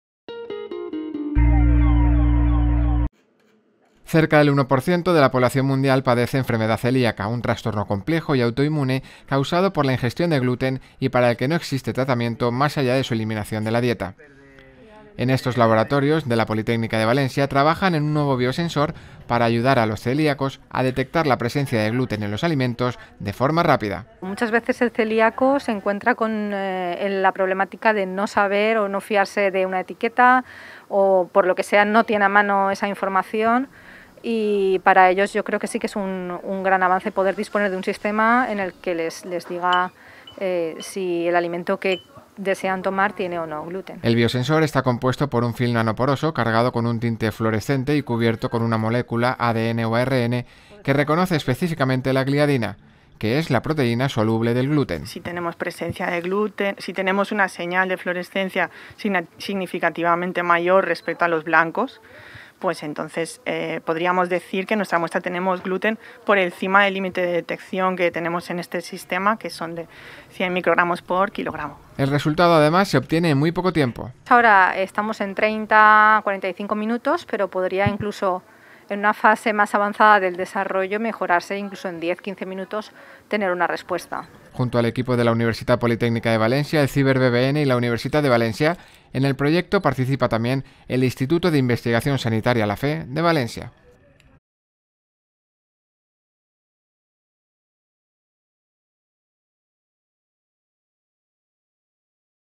Nuevo dispositivo para detección más eficaz de gluten - Noticia @UPVTV, 26-05-2021
Cerca del 1% de la población mundial padece enfermedad celíaca, un trastorno complejo y autoinmune causado por la ingestión de gluten y para el que no existe tratamiento más allá de su eliminación de la dieta. En España cada 27 de mayo, se conmemora el Día Nacional del Celiaco para dar a conocer la enfermedad y dar visibilidad y apoyo a las personas celiacas. En este contexto, un equipo de la Universitat Politècnica de València, la Universitat de València, el Instituto de Investigación Sanitaria La Fe y el CIBER-BBN trabaja en un nuevo dispositivo para la detección rápida de gluten.